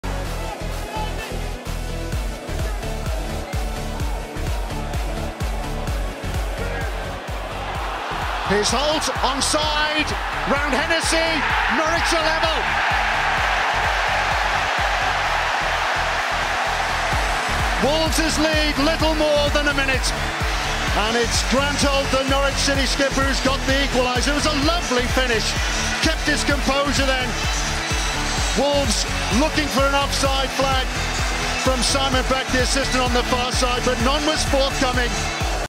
諾域治名人堂格蘭賀治英超入球。
英超球會諾域治(Norwich City)名人堂及前隊長格蘭賀治(Grant Holt)在2011/2012球季主場對狼隊的入球。諾域治結果由格蘭賀治梅開二度，反勝2比1而全取3分。諾域治至今都未能夠找尋到格蘭賀治的接班人。
此影片由英超官方頻道錄影下來，並非屬於本人，只希望和球迷分享。